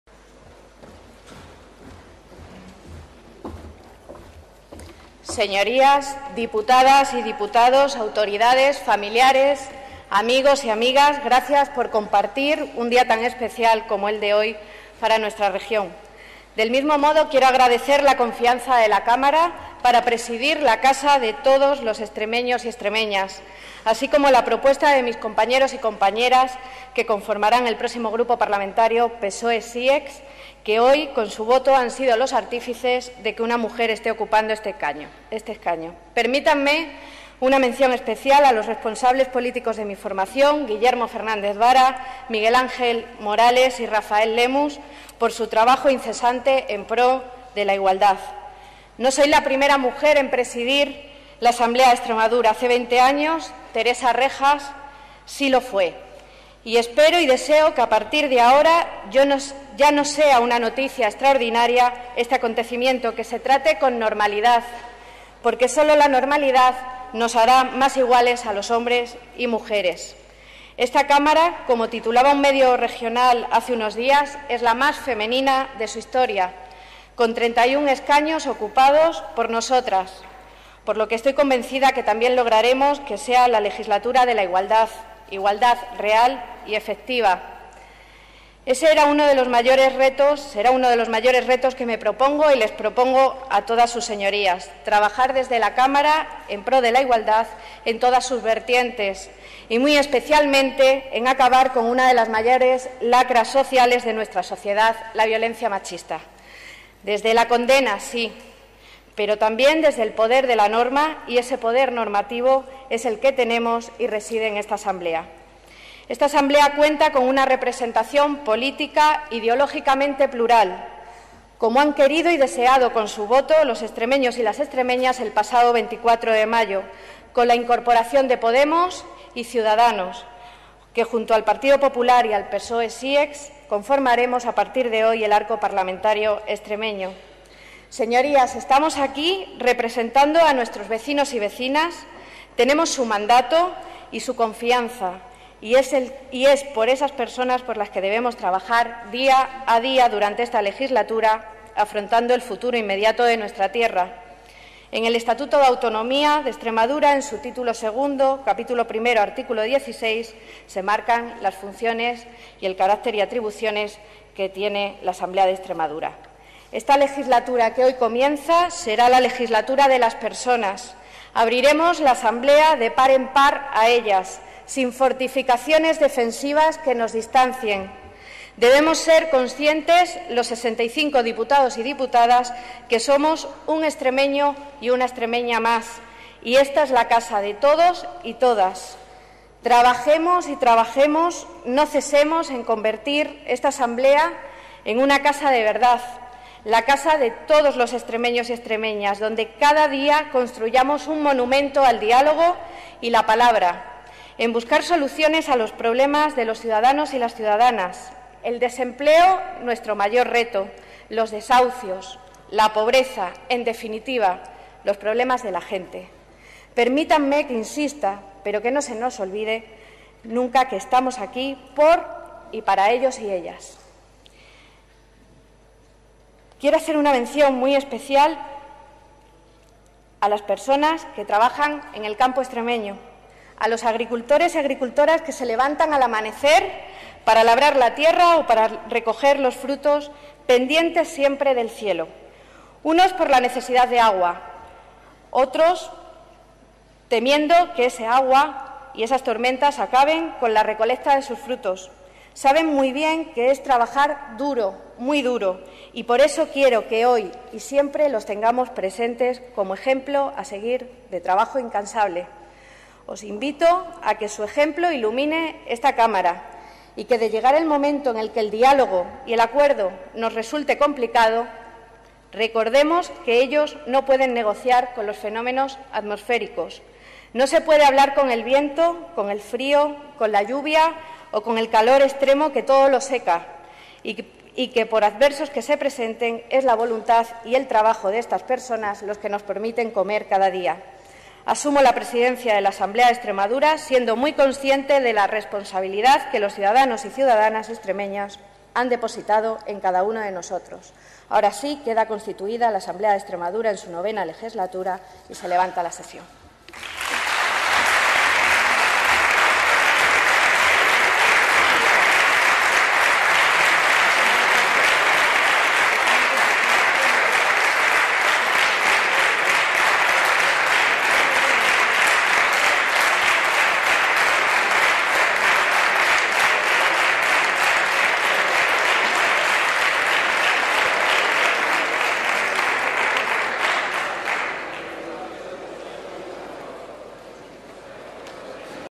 Discurso de Excma. Sra. D. Blanca Martín Delgado, 23 de junio de 2015
La presidenta de la Asamblea de Extremadura en su IX Legislatura, doña Blanca Martín Delgado, dirige unas
palabras a los miembros de la Cámara, declara constituida la misma y levanta la sesión cuando son las 13.54
horas.